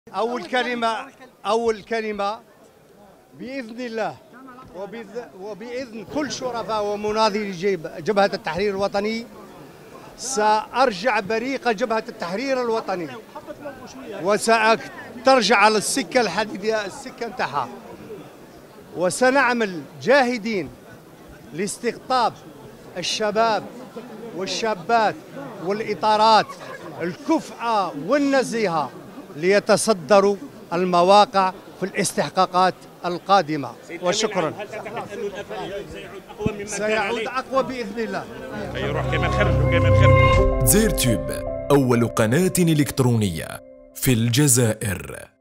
.. تعرف على أول تصريح لعبد الكريم بن مبارك بعد انتخابه أمينا عاما للأفلان بالإجماع..
شااااهد.. "بإذن الله نرجع البريق تاع حزب جبهة التحرير الوطني".. تعرف على أول تصريح لعبد الكريم بن مبارك بعد انتخابه أمينا عاما للأفلان بالإجماع..

البث الحي / البث المباشر قناة دزاير توب